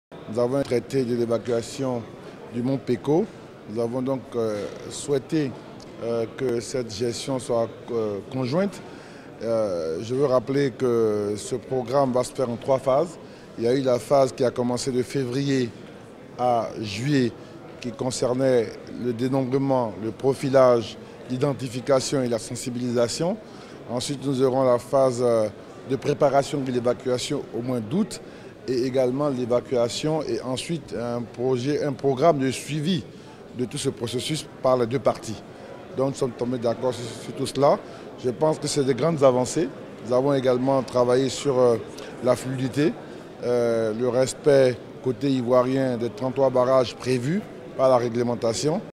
Traité d’Amitié et de Coopération C.I ; BF: Réaction hamed bakayoko
4ème Conférence au sommet du Traité d’Amitié et de Coopération: Réaction hamed bakayoko .